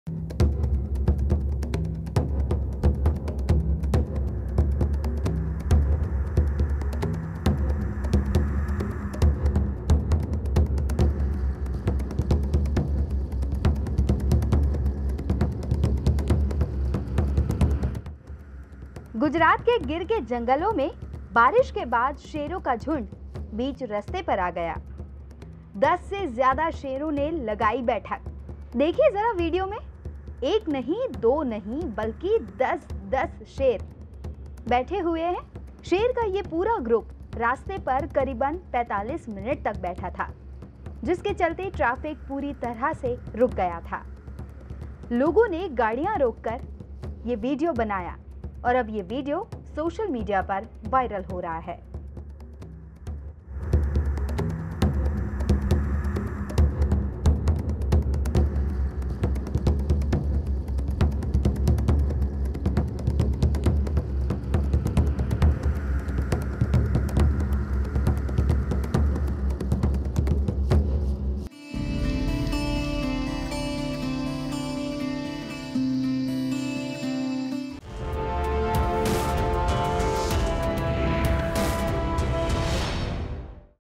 देखिए कैसे शेरों ने किया रास्ता बंद ? | Gujarat Tak
Lions sitting on road..